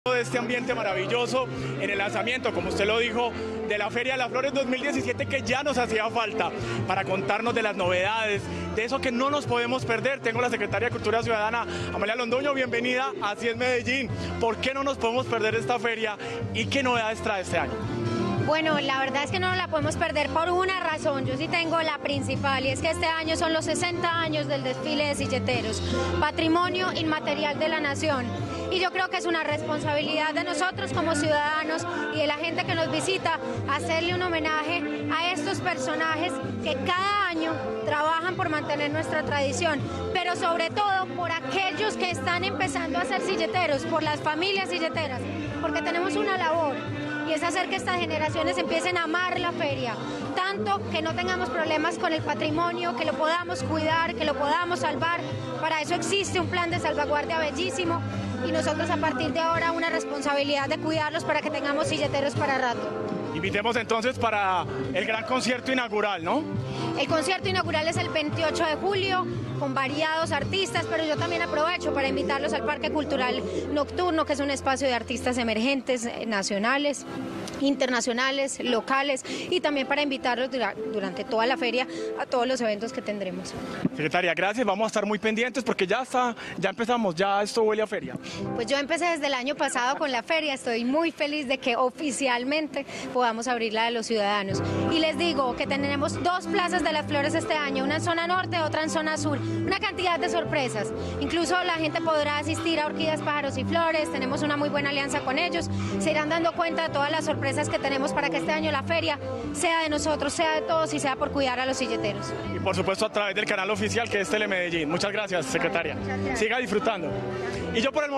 Novedades que tendrá la Feria de las Flores para este año [Noticias] - Telemedellín
La secretaria de Cultura de Medellín, Amalia Londoño habló sobre las novedades que tendrá la Feria de las Flores para este año.

El evento más destacado, el Desfile de Silleteros cumple 60 años de historia.